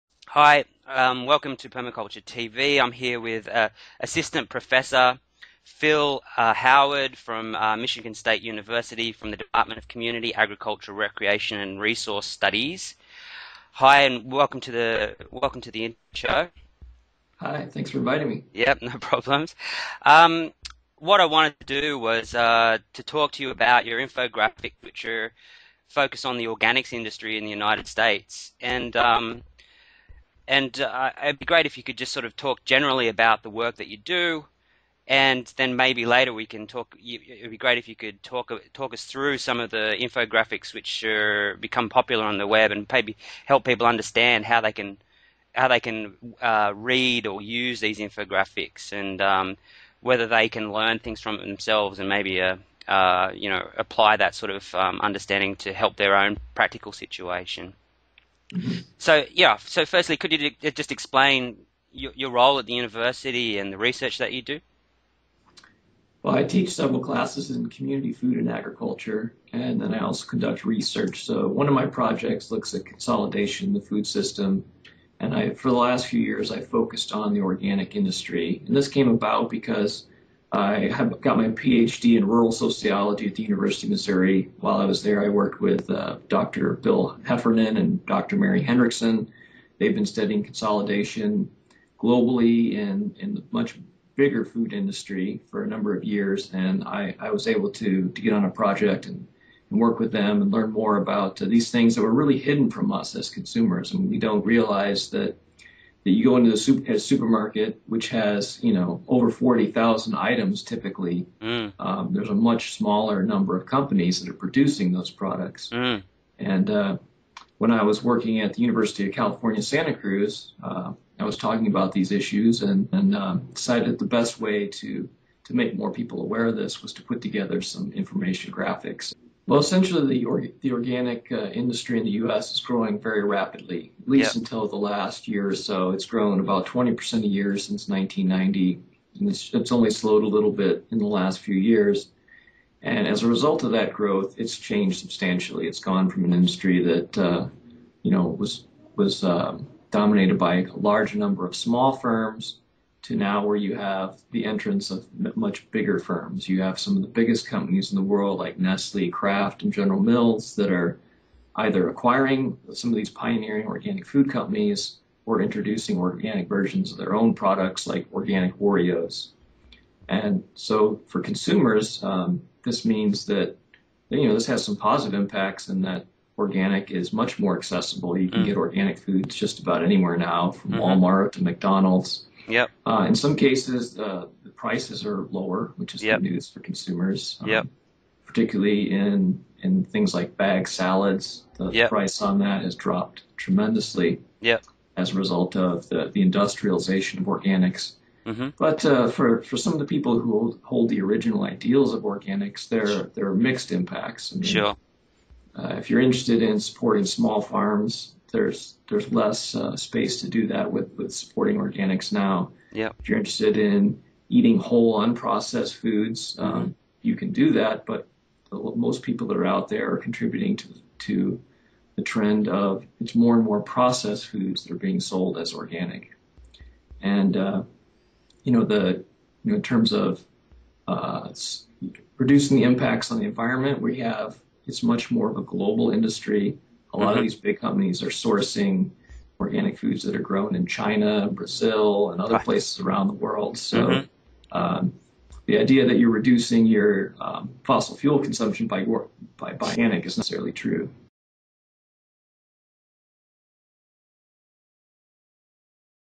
Organics Industry USA - Prof Phil Howard, MSU
Dr Phil Howard introduces his research into the structure and dynamics of the USA Organics industry. In relation to the industrialisation of organics, the corporate take-over and consolidation and the overall growth of the organics market.